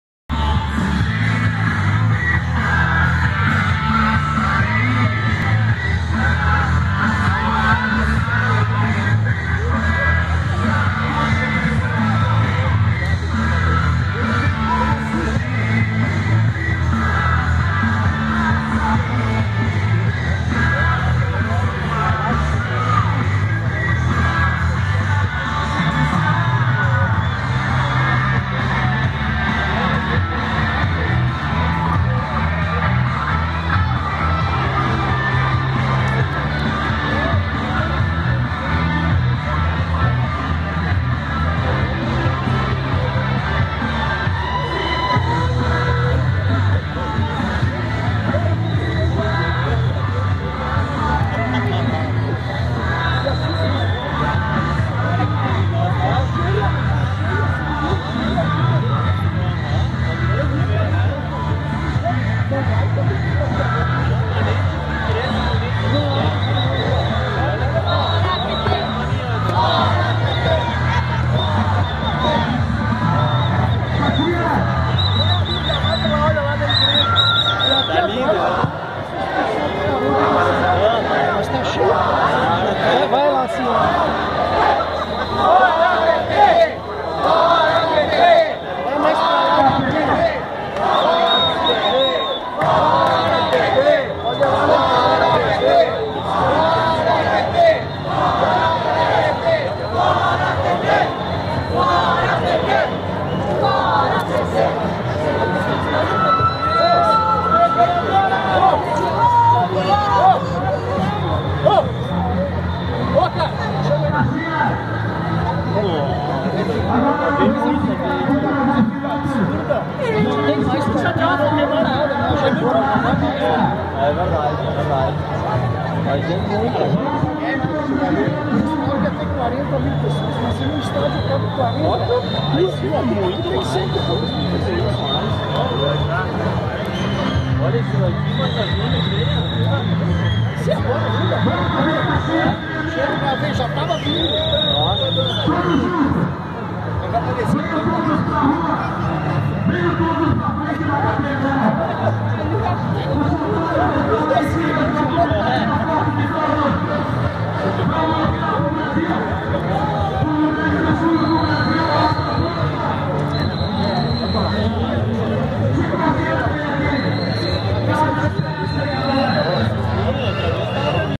Protesto contra governo federal reúne milhares em Caxias
Imagens: Elizangela Beto/Rádio Caxias